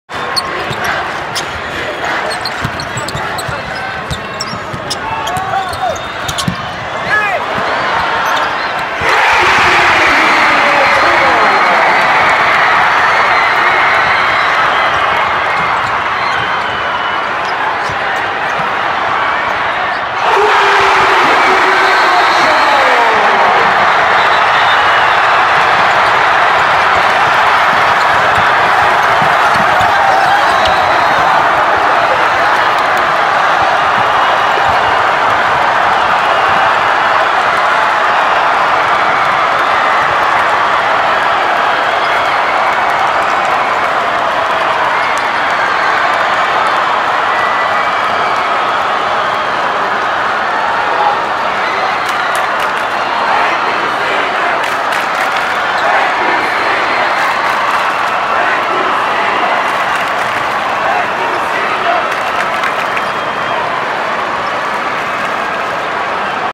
Stunning finish: Watch as Prescott beats Valders for D3 state title
Prescott's three-point buzzer-beater stuns Valders in Saturday's WIAA Division 3 state championship game at the Kohl Center in Madison.